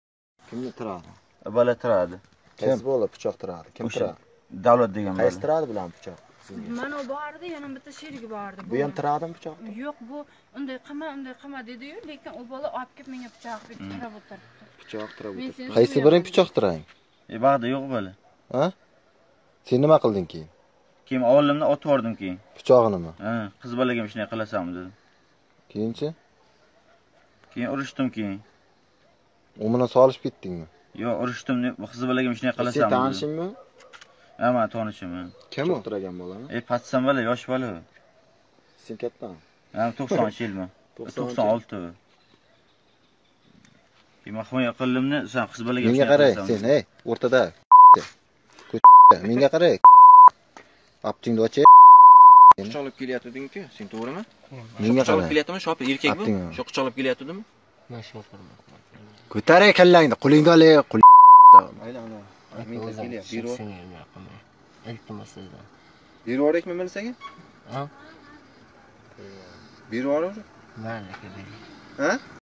В МВД прокомментировали самосуд над молодыми людьми в Самарканде
Против мужчин, устроивших самосуд, возбудили уголовное дело.